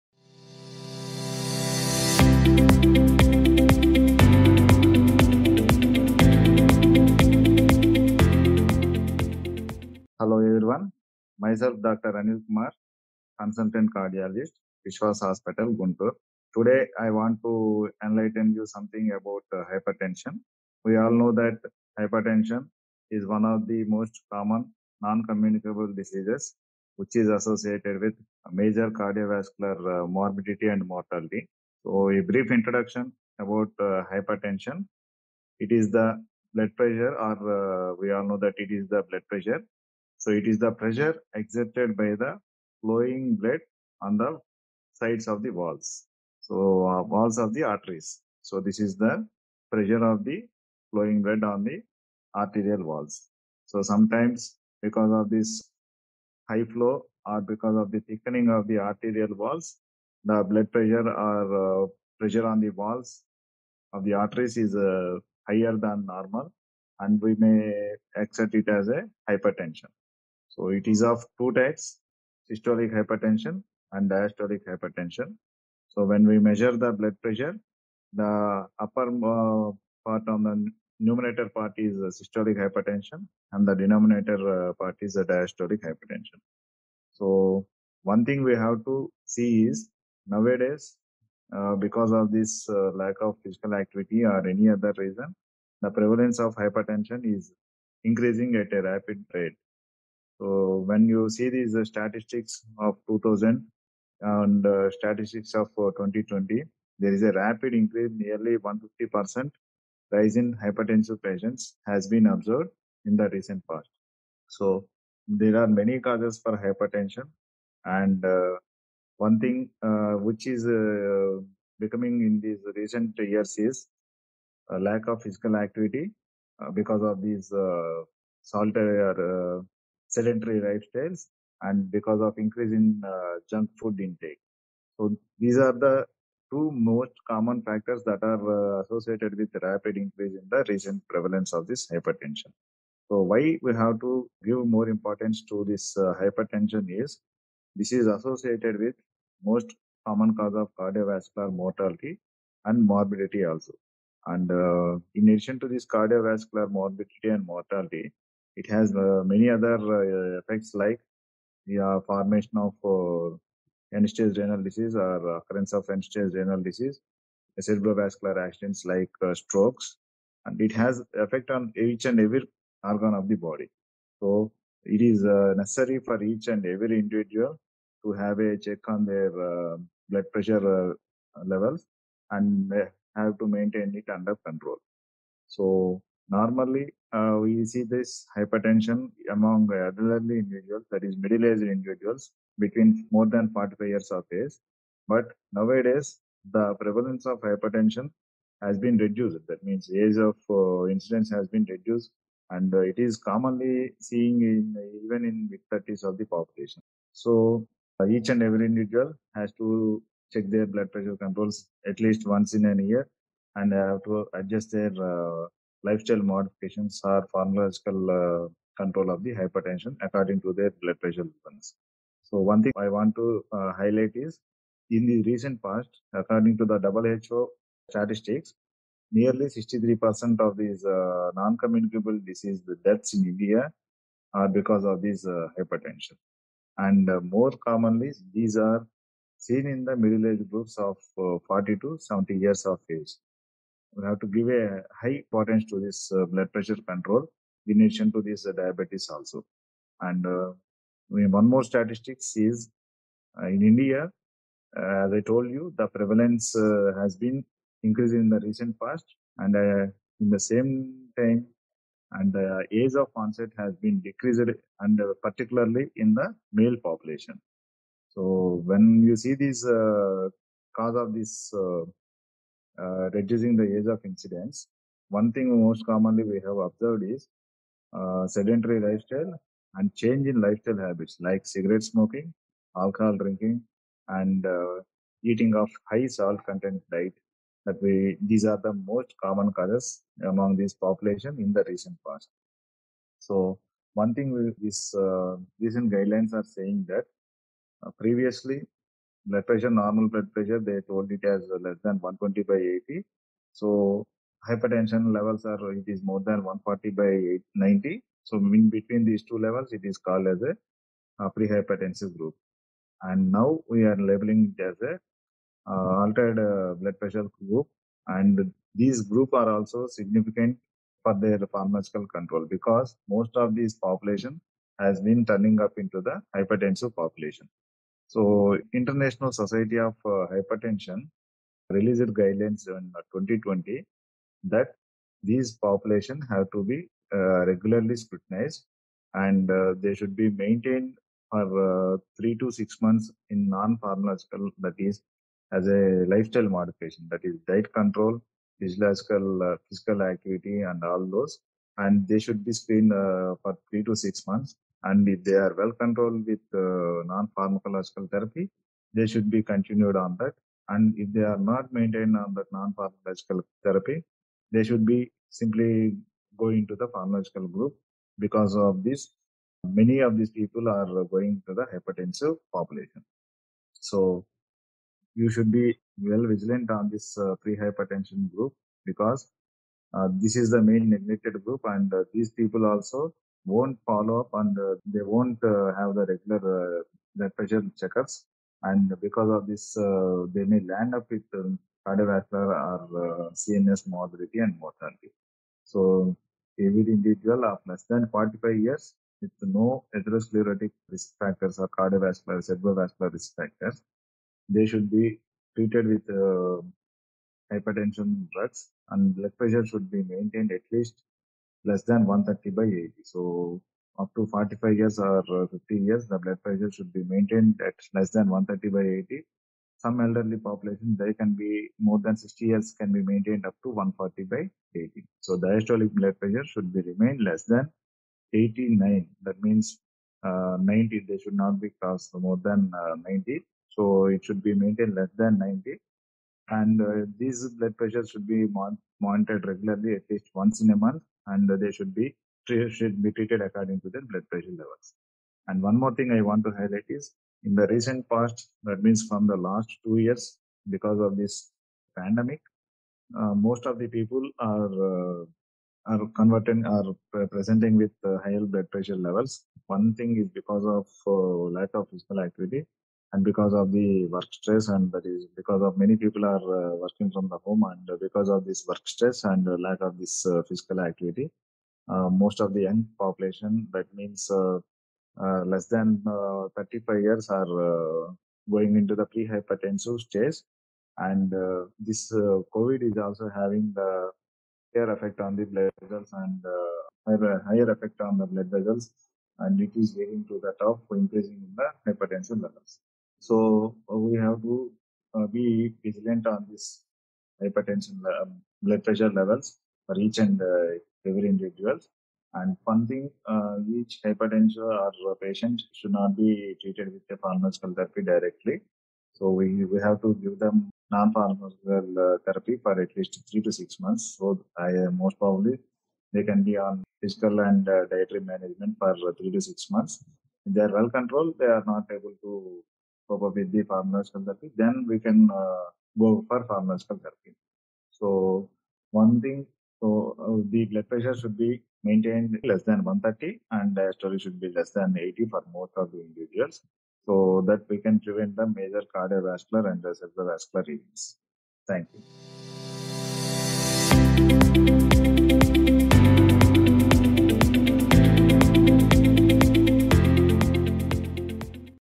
Latest Update on Hypertension | Dr. Anil Kumar
As per WHO, nearly 63% of total deaths in India are due to non-communicable diseases, of which 27% are attributed to CVD which affects 45% of people in the 40-69 age group. Raised blood pressure is among the most important risk factors for CVDs which remains poorly controlled due to low awareness about hypertension, lack of appropriate care through primary care, and poor follow-up of the patients.   In India, the prevalence has also gone up over 30 years to 38% in men and 32% in women.  Despite that, nearly 51% of the men and 41% of women with hypertension were unaware of the condition. Listen to the expert to know more on the latest updates in Hypertension.